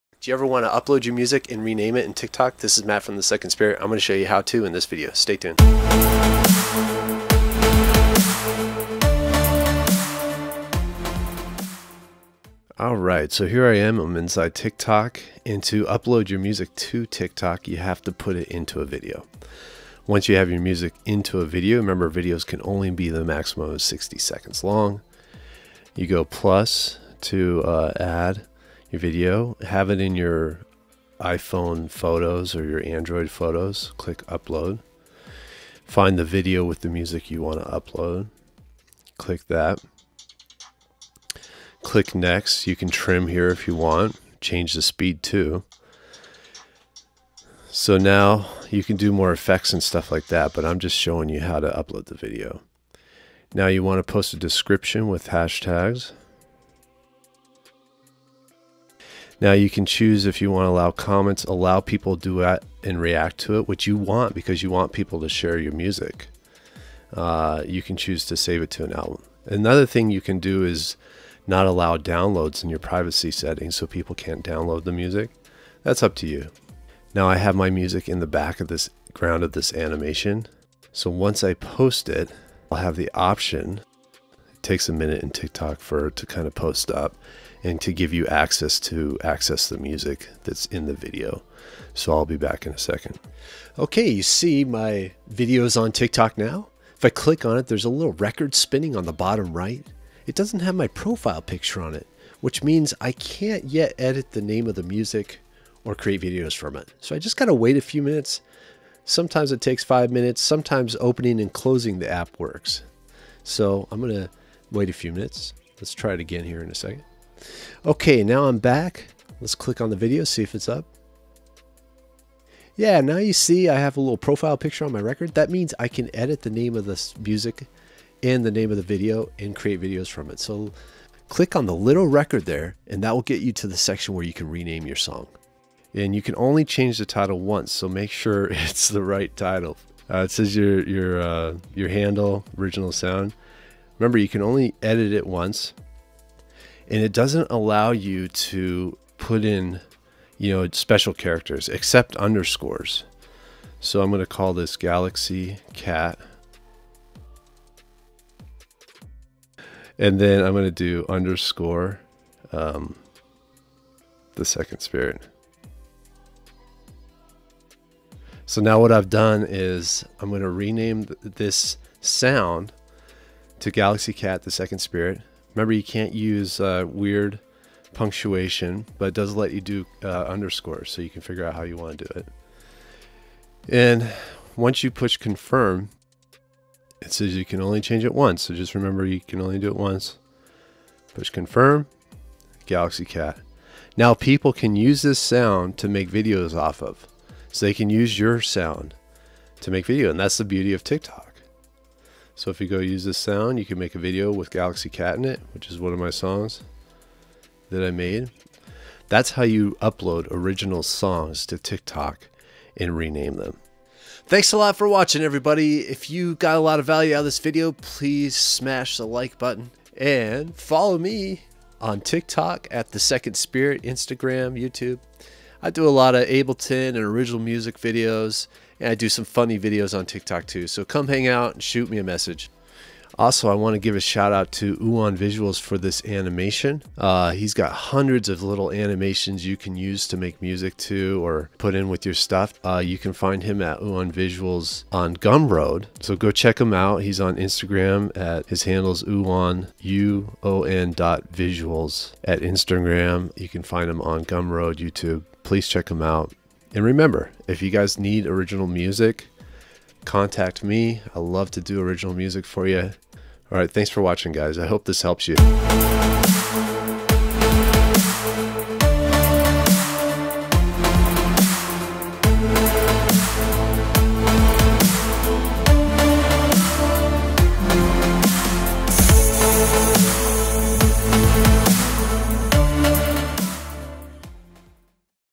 TikTok Upload & Rename Music on Tik Tok without a Distributor - How To Step by Step
TikTok is a great app for musicians.  It is a video app which allows people to make videos to your music. In this tutorial, I teach you how to upload and rename your original music on TikTok without a distributor.  If someone makes a viral video off your song, that means your song will go viral.

Warning: if you upload music this way, you may gain lots of brand awareness and attention but will probably not receive royalties.  To receive royalties, you would need to distribute your music to TikTok using a distributor like Distrokid:

Walkthrough:
⚡️To put your music into TikTok, so people can make videos from it, you must add your music to a video first.  The most used video length is 15 seconds, but videos can be up to 60 seconds long.  After you have your music in a video you can upload it to TikTok.

⚡️You must use your phone to upload videos to TikTok, so have your video on your phone, then open the TikTok app.

⚡️Push the uploads in the bottom right to upload your video with your music on it. Once you upload your video you can edit your video and put effects in it.

⚡️You can post a caption / description with hashtags. The caption / description can be 150 characters only. A lot of people add the hashtag for or or so your video can come up in the "For You" page that everyone sees on TikTok.

⚡️You can also choose to allow people to comment or duet and react to your video.  I would allow this because it gives people more options to share and play with your music.  In the privacy settings you can choose if people can download your video.   After that you can click through to next imposture video.

⚡️Once you post your video, you'll see it in your videos on TikTok.  Click the video did you want to rename the music too that has your music in it. Once you're in the video look at the bottom right you'll see a little record spinning.  Is your profile picture in the record or is it blank?   If you don't see your profile picture spinning in the record you can't edit the music in the video yet. You might have to wait a few minutes or try closing and reopening the app.

⚡️Check again in see if your profile picture is in the record. once the profile picture is spinning in the record that means you can edit and rename your song. To edit and rename your song click the record in push edit. YOU CAN ONLY EDIT AND RENAME YOUR SONG ONCE so make sure you spell it right and you have the name you want.

TikTok doesn't allow you to put in weird characters for your songs. The one character it did let me put in is an underscore, but hyphens didn't work.

⚡️Rename your song and click done.   Now people can make videos to your song.

Please check out UON Visuals huge library of affordable animations you can add your music to upload to TikTok!